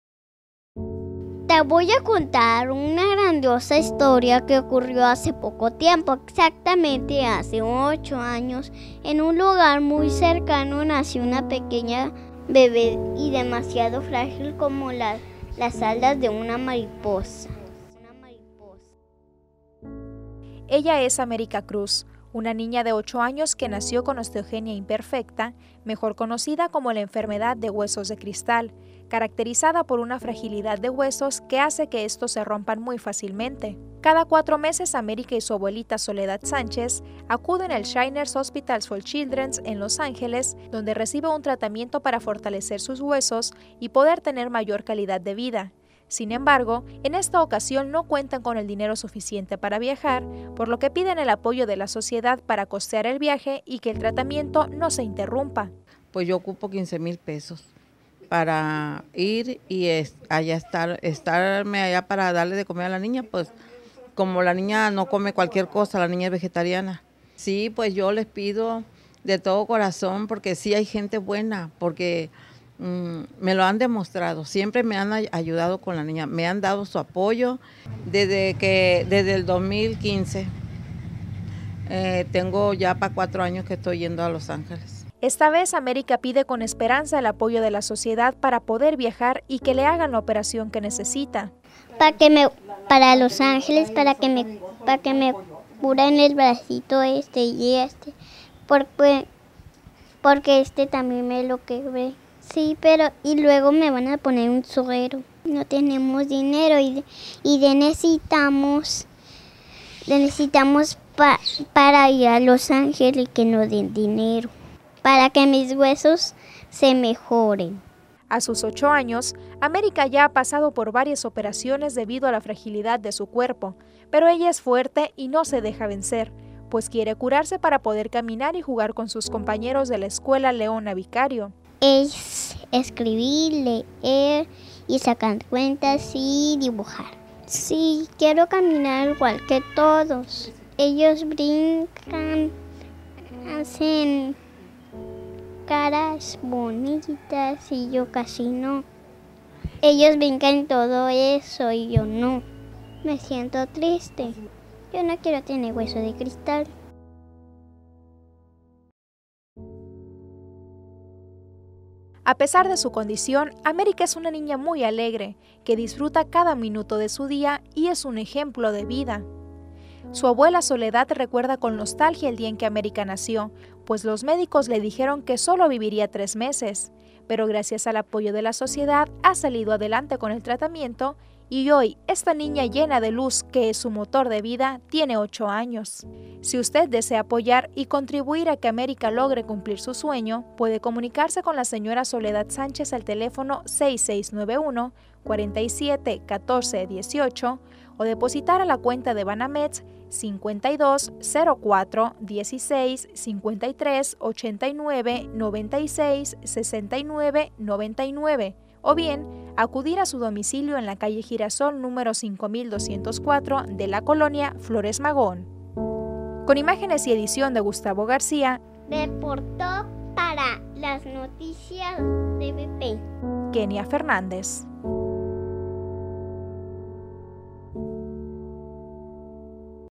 América pide ayuda para cumplir su sueño
Es una niña que padece la enfermedad conocida como "huesos de cristal" y para continuar su tratamiento y lograr caminar debe viajar a Los Ángeles, lamentablemente no tiene el dinero suficiente, ¡usted puede ayudarla!